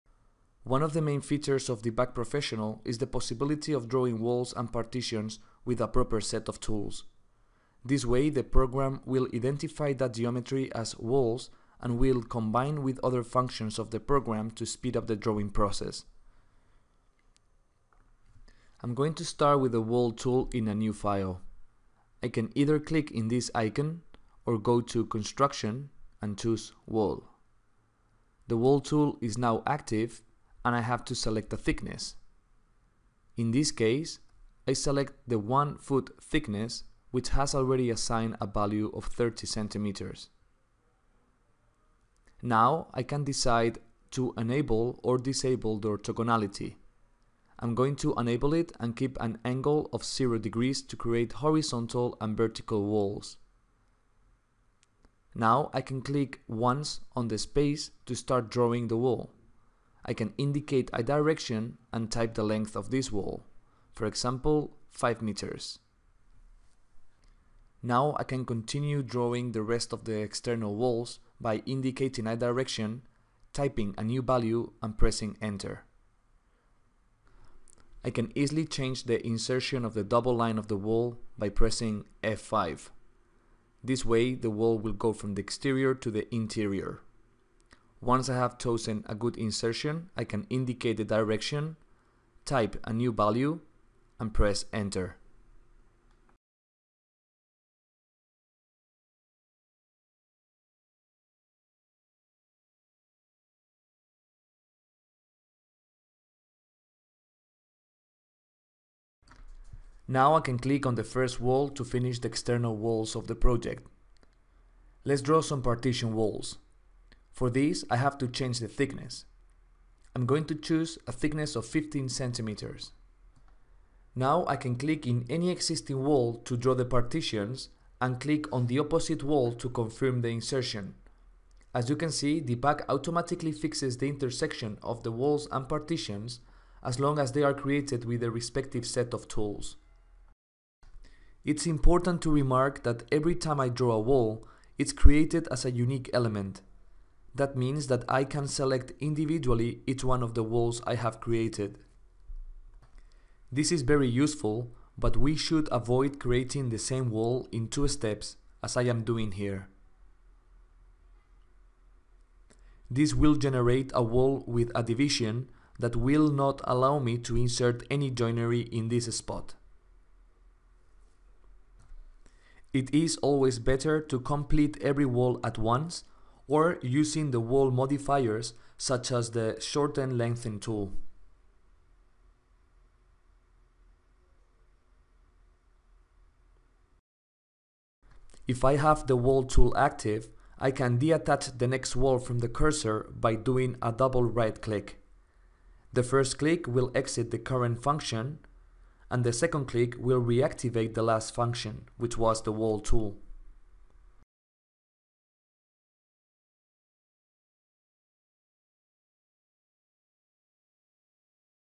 #1 Wall tool | Construction
This function lets you draw the walls and partitions of your project with a specific thickness. Learn how easy and fast it is to draw a floor-plan with DIBAC CAD.